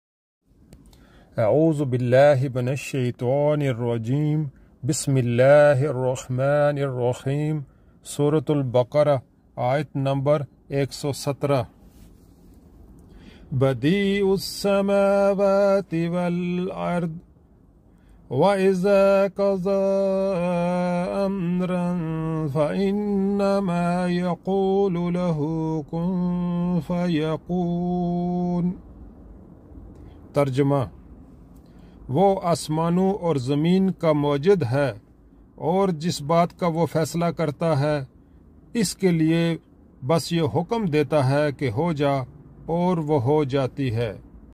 " سورۃ البقرۃ: آیت نمبر 117"
اہم احکام 📝
        سورة البقرة - آیت نمبر 117🌷

بَدِیۡعُ السَّمٰوٰتِ وَ الۡاَرۡضِ ؕ وَ  اِذَا قَضٰۤی اَمۡرًا فَاِنَّمَا یَقُوۡلُ لَہٗ  کُنۡ فَیَکُوۡنُ ۔

وہ آسمانوں اور  زمین  کا موجد ہے ، اور جس  بات  کا وہ  فیصلہ  کرتا ہے ، اس کے لیے بس یہ  حکم  دیتا ہے کہ ’’ ہو جا ‘‘ اور وہ ہو جاتی ہے ۔
قرآن مجید اور احادیث مبارکہ کے لیے اقراء پروجیکٹ کو جوائن کریں اور دین کی دعوت عام کرنے میں ہمارا ساتھ دیں ۔سورة_البقرة